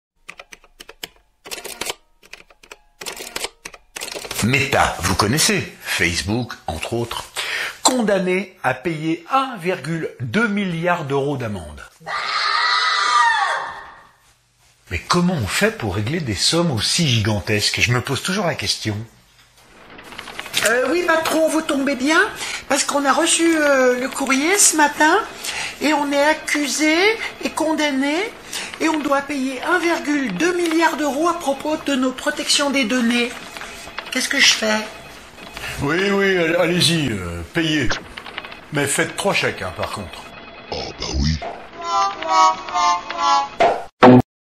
Comment règle t-on 1,2 milliard d'amende ?
Je me pose la question a chaque fois suite a ses amendes records ??

Merci d'avance  ;-)